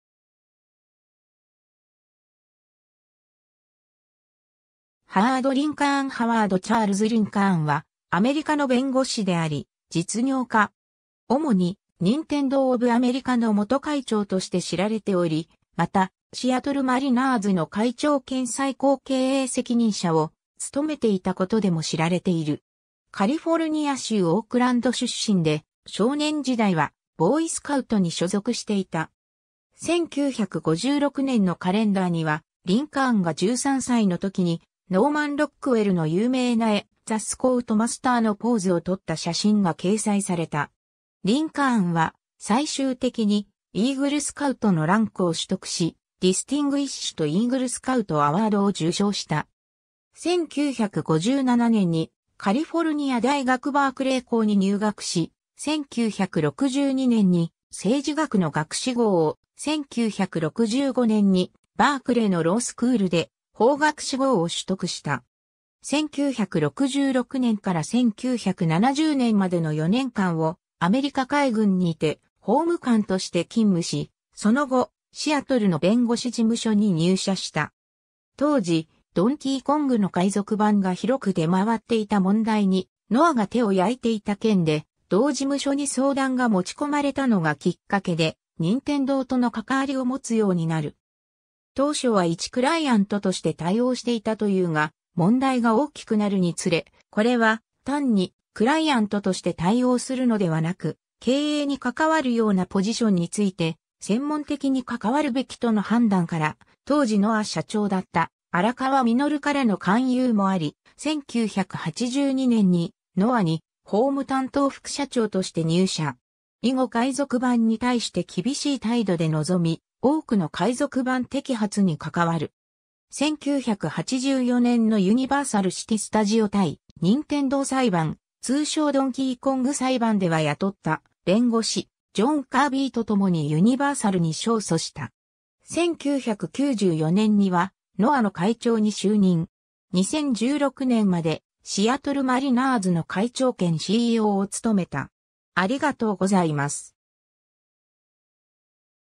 ハワード・リンカーン
シアトル・マリナーズ関連人物
ハワード・リンカーン（左） ハワード・チャールズ・リンカーン（Howard Charles Lincoln, 1940年2月14日 - ）はアメリカの弁護士であり実業家。
主にNintendo of America（NOA、任天堂のアメリカ現地法人）の元会長として知られており、また、シアトル・マリナーズの会長兼最高経営責任者を務めていたことでも知られている（2013年9月19日に任天堂社長の山内溥が亡くなるまでは、彼の代理人として参画）。
 カリフォルニア州オークランド出身で、少年時代はボーイスカウトに所属していた。
1956年のカレンダーには、リンカーンが13歳の時にノーマン・ロックウェルの有名な絵「The Scoutmaster」のポーズをとった写真が掲載された。
リンカーンは最終的にイーグル・スカウトのランクを取得し、Distinguished Eagle Scout Awardを受賞した。
 1957年にカリフォルニア大学バークレー校に入学し、1962年に政治学の学士号を、1965年にバークレーのロースクールで法学士号を取得した。
1966年から1970年までの4年間をアメリカ海軍にて法務官として勤務し、その後、シアトルの弁護士事務所に入社した。
当時『ドンキーコング』の海賊版が広く出回っていた問題にNOAが手を焼いていた件で同事務所に相談が持ち込まれたのがきっかけで任天堂とのかかわりを持つようになる。
当初は一クライアントとして対応していたというが、問題が大きくなるにつれ「これは単にクライアントとして対応するのではなく、経営に関わるようなポジションに就いて専門的に関わるべき」との判断から、当時NOA社長だった荒川實からの勧誘もあり、1982年にNOAに法務担当副社長として入社。
以後海賊版に対して厳しい態度で臨み、多くの海賊版摘発に関わる。
1984年のユニバーサル・シティ・スタジオ対任天堂裁判、通称ドンキーコング裁判では雇った弁護士ジョン・カービィと共にユニバーサルに勝訴した。
 1994年にはNOAの会長に就任。
2016年までシアトル・マリナーズの会長兼CEOを務めた。